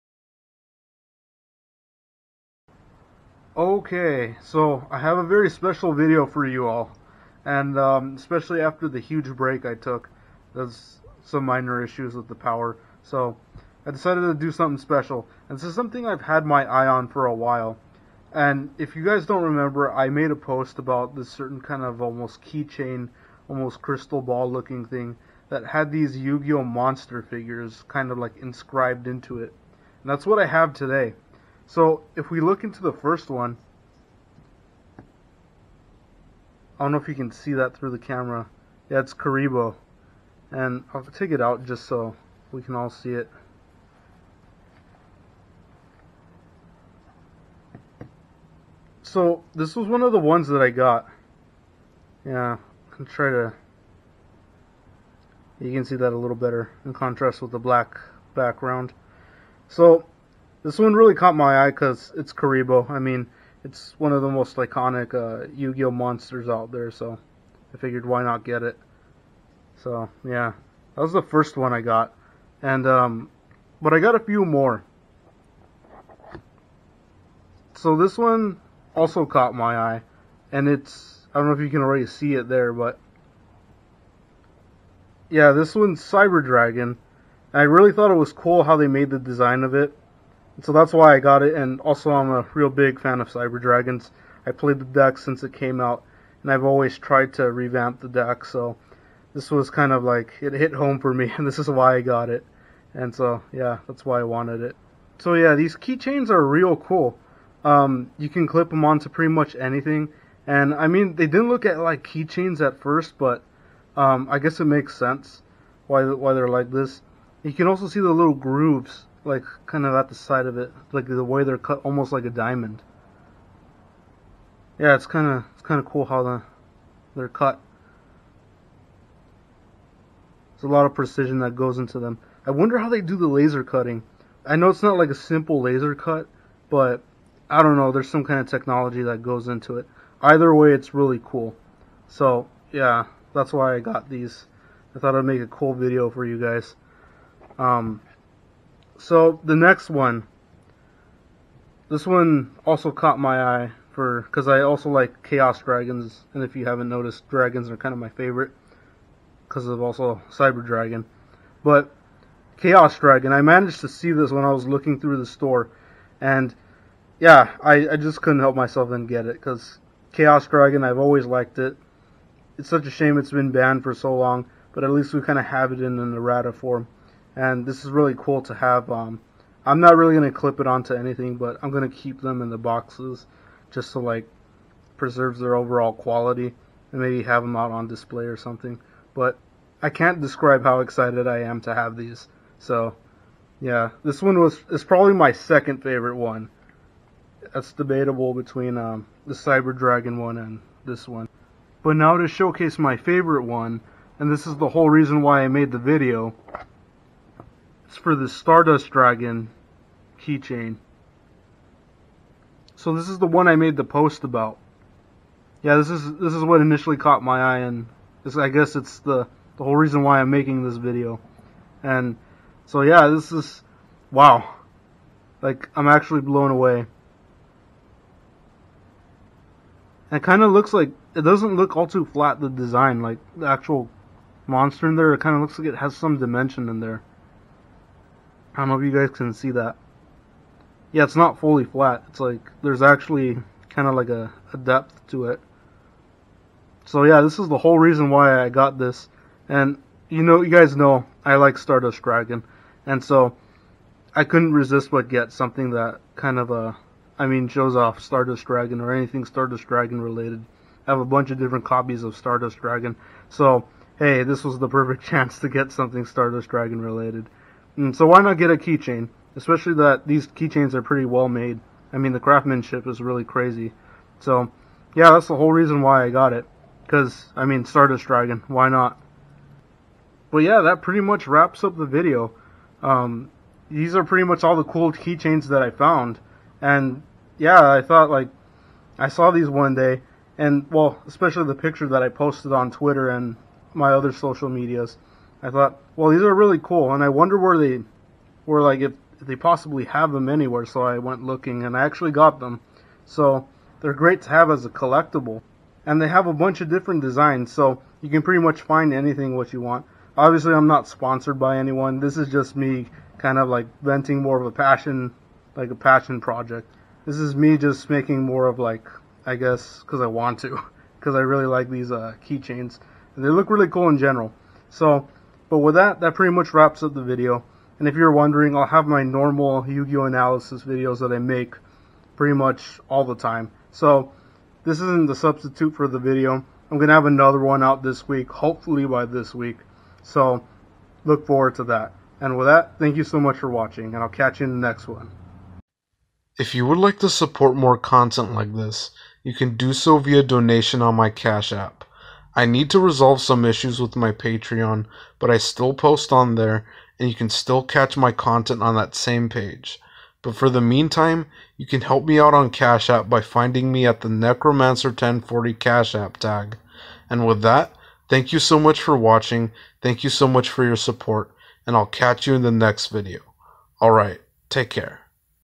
Yugioh! Laser Cut Crystal Key Chains!
Hey everyone! I'm finally back after a bit of a hiatus and I'm back with an unboxing video. I'm going to unbox this set of yugioh keychains that I bought online. These keychains were very unique to me and I decided to get them since I thought they were interesting. Thank you so much for your support, I hope you enjoy the video, and I'll catch you in the next one. Take care.

$Necromancer1040